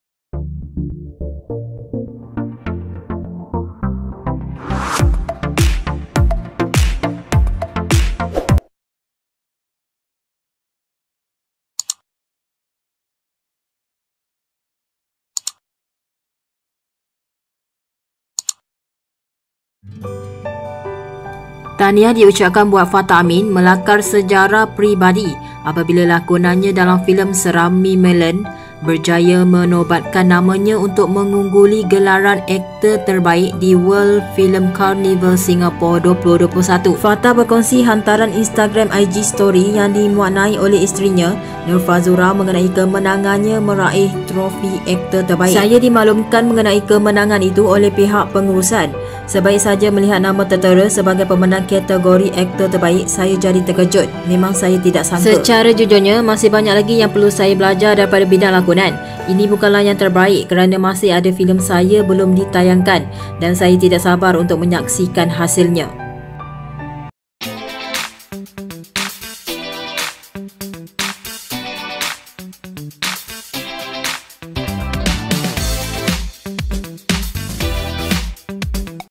TAHNIAH! Rezeki..Fattah Amin Tak Sangka. Perkara Itu Dikongsi Oleh Fazura Sendiri
Fattah amin telah dinobatkan sebagai aktor terbaik melalui filem seram miimaland di World Film Carnival Singapore 2021. Perkara tersebut diketahui melalui pihak pengurusannya.

Kisah artis popular tanah air dan semasa